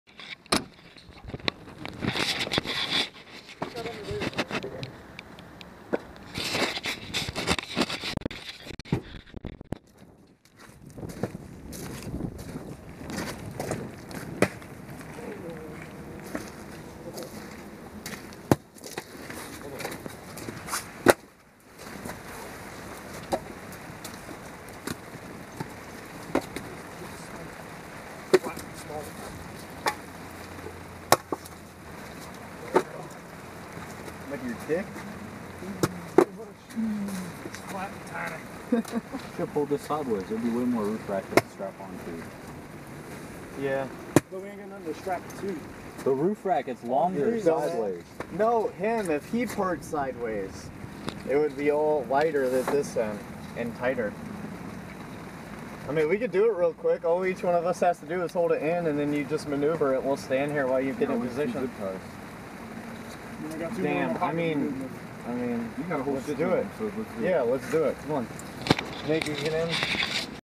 Rig Shelter Pt.1
Pulling the Jeeps together with a tarp ad chopping up wood for survival and stuff.